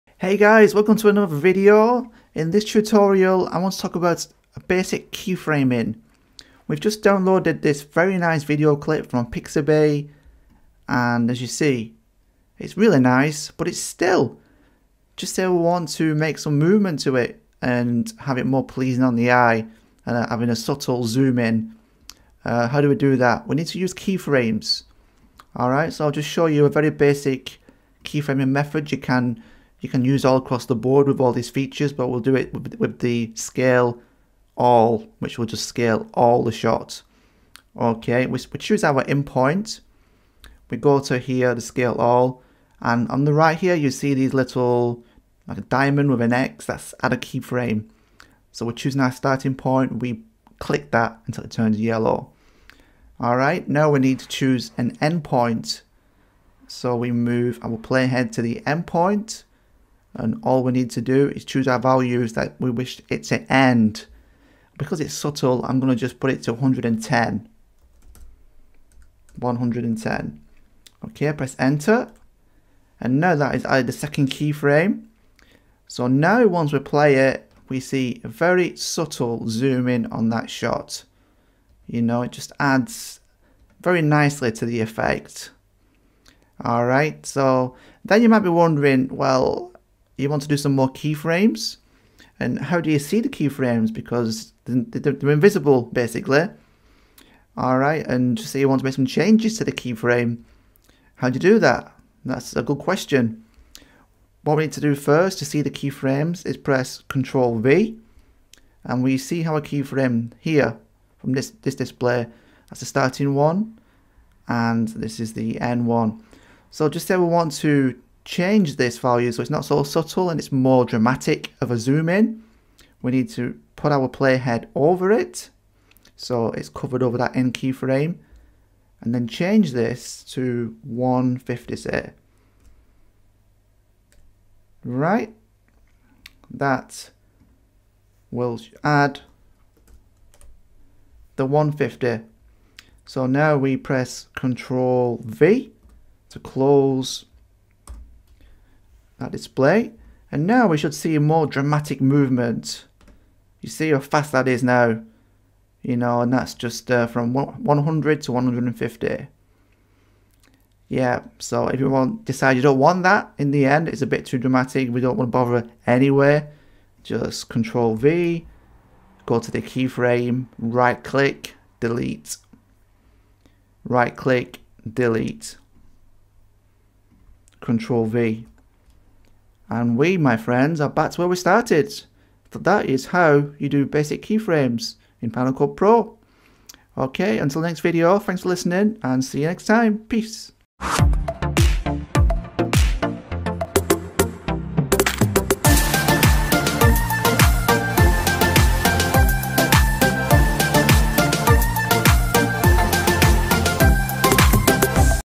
BASIC KEY FRAMING IN Final Cut Pro X // Key Frame a 'ZOOM IN'
In this tutorial we show how a basic key frame movement of a zoom in on a clip.